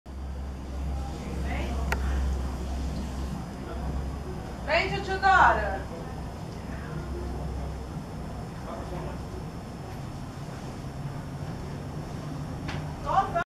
Casal de Bico de Lacre Namorando
Flagra  Casal de Bico de Lacre namorando . Vídeo curto .comecei filmar saíram voando
Natureza Perfeita
Passaros
Bico de Lacre